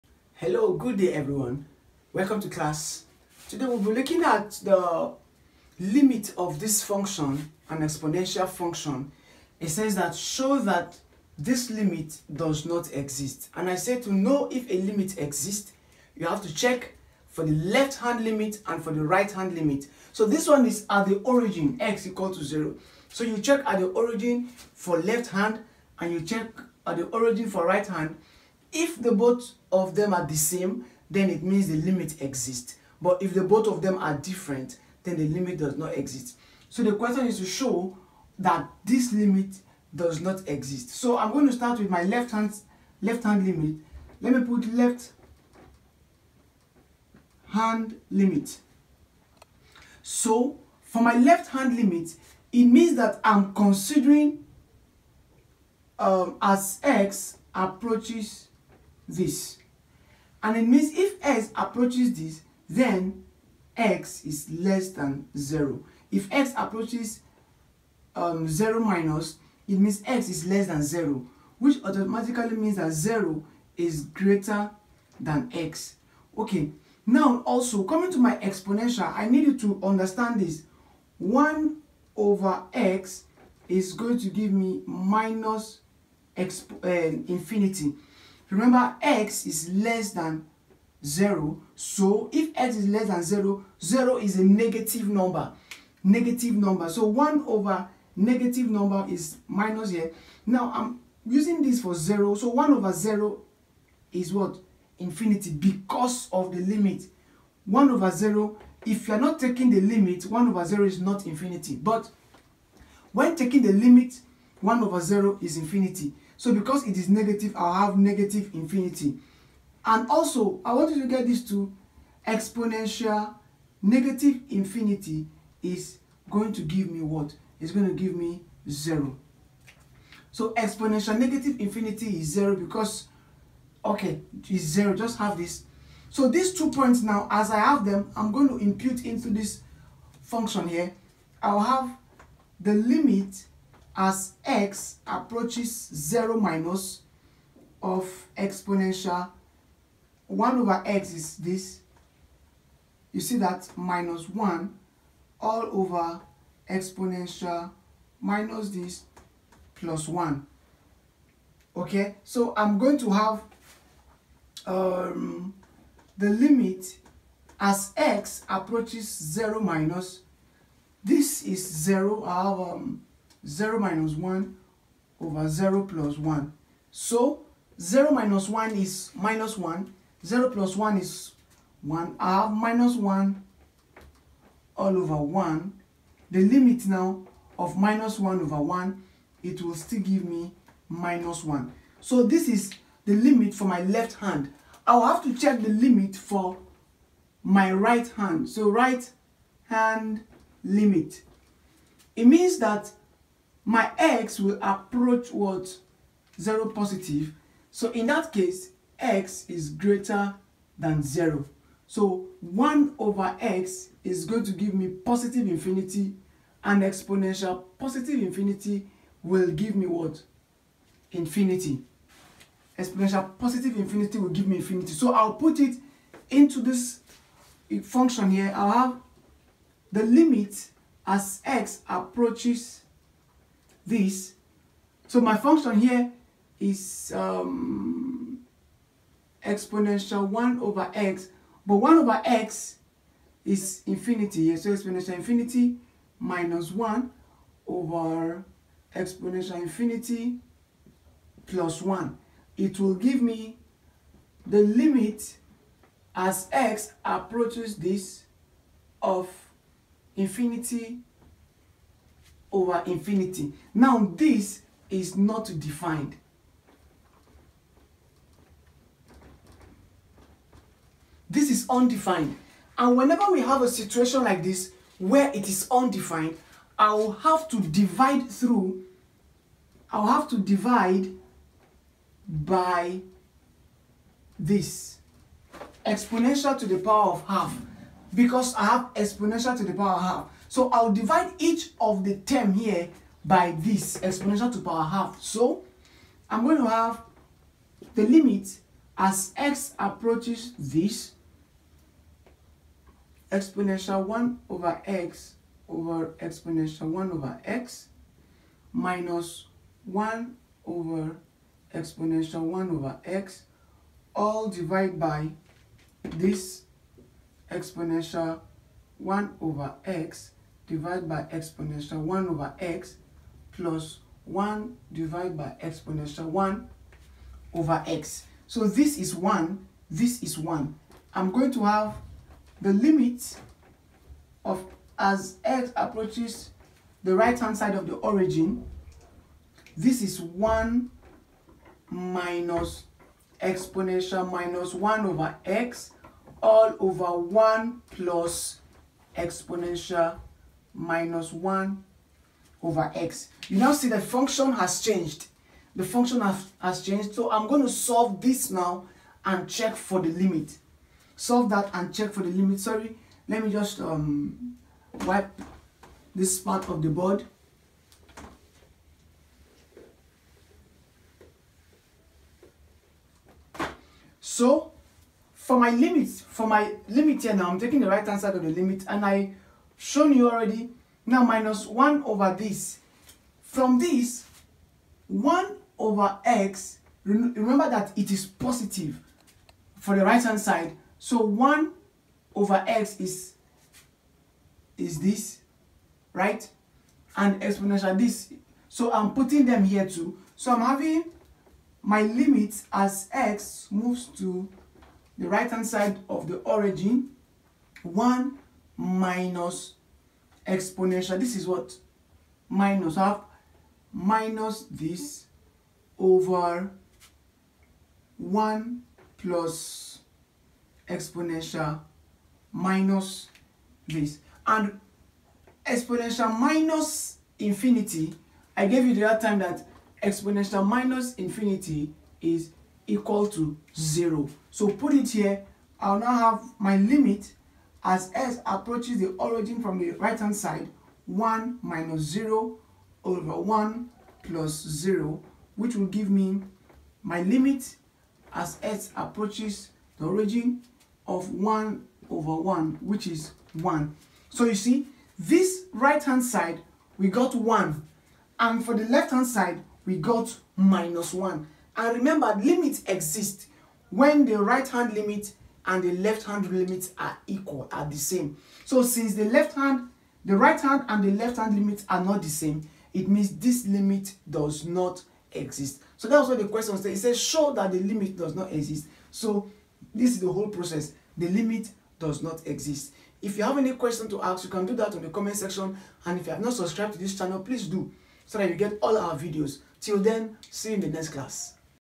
What to do when exponential function gives infinity (right and left limit)
Do this whenever you have an undefined limit of an exponential function